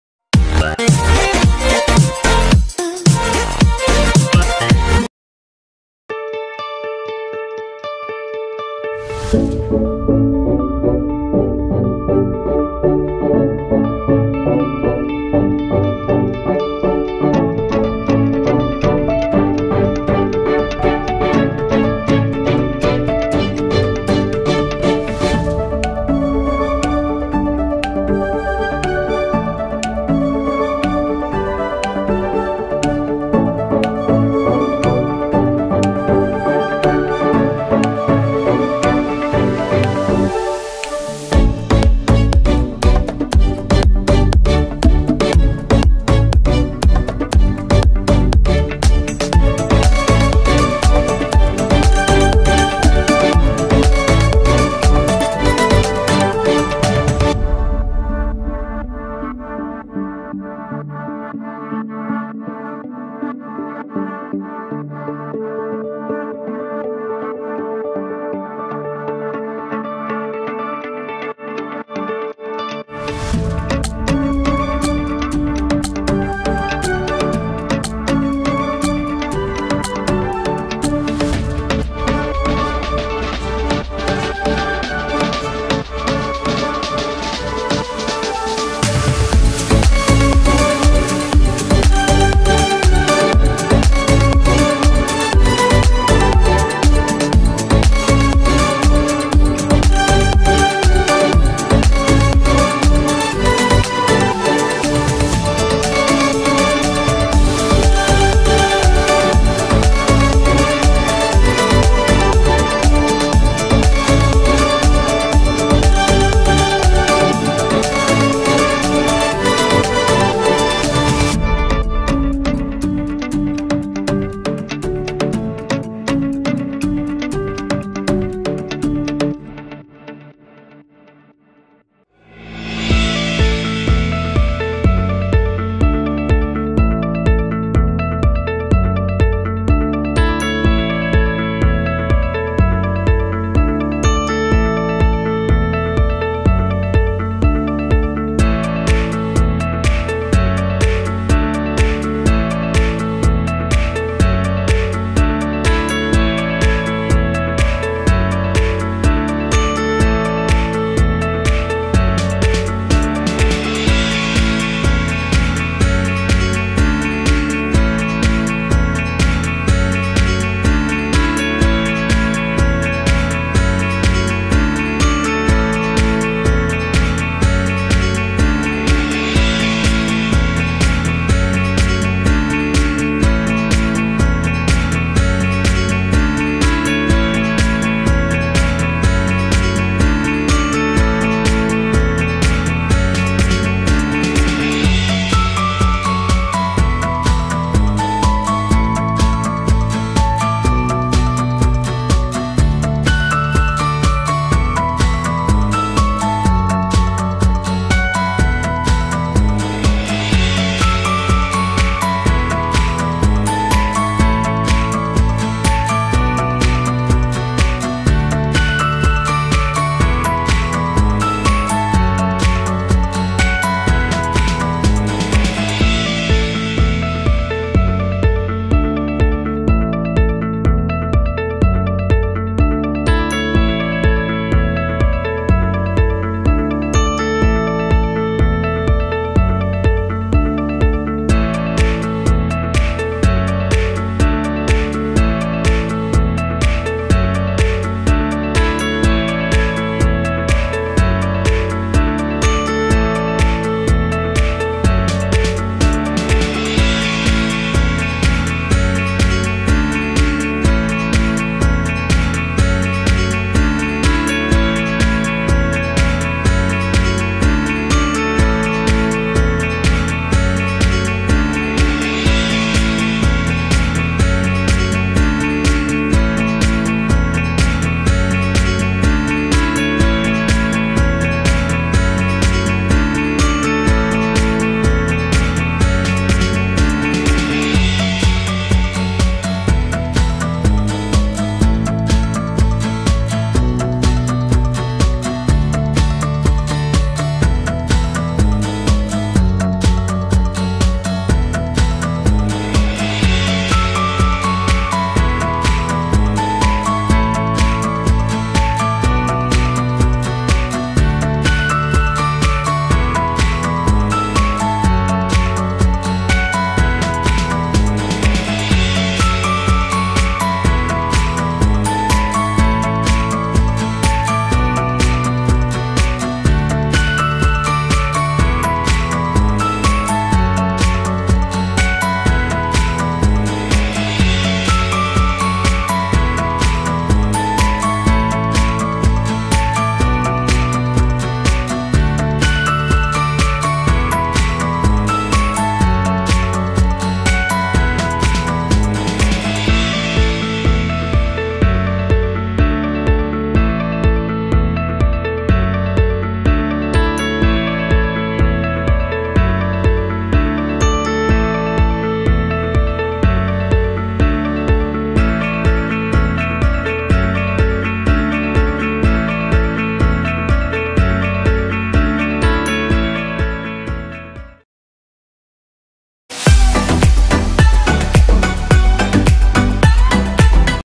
Best out of waste | newspaper crafts | Home decoration ideas with newspaper
Reuse old newspapers
Home decoration , wall decoration ideas

Materials needed :-
1. One rectangular cardboard
2. Old newspapers
3. Glue + water
4. Acrylic paint ( black )
5. String and tape
6. Cotton ( optional )